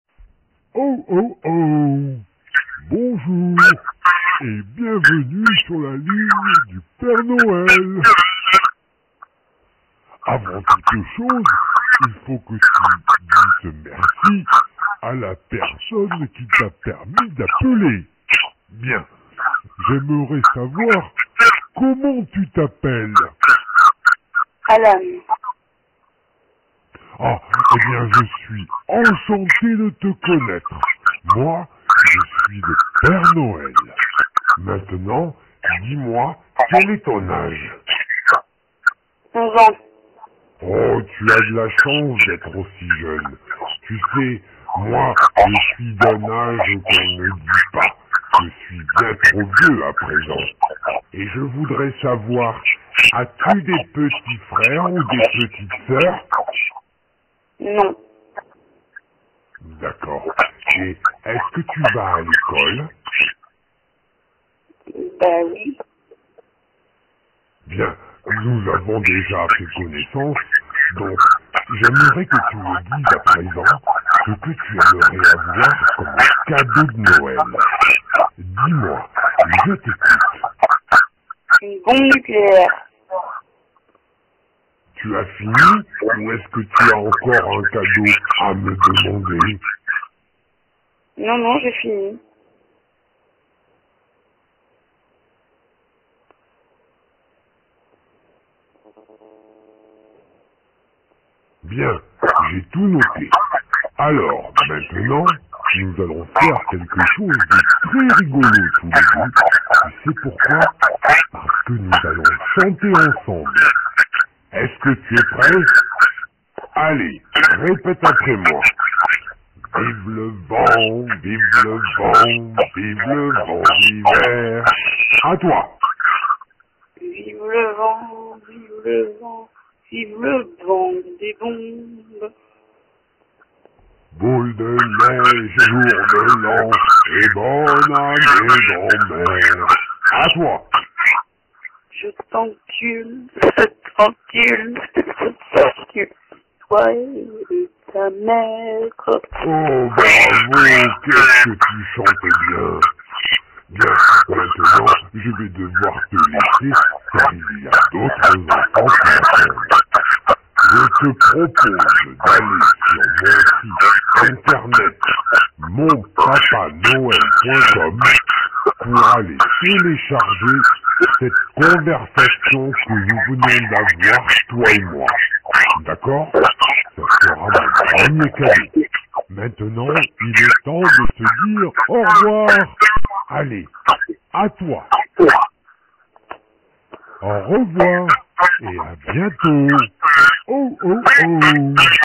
Message Personnalisé du Père Noël à Alan - Regardez la vidéo le 01-08-2016 a 23:43:33
Ecoutez Alan discuter avec le Père Noël, chanter avec lui et lui donner sa liste de cadeaux de Noël.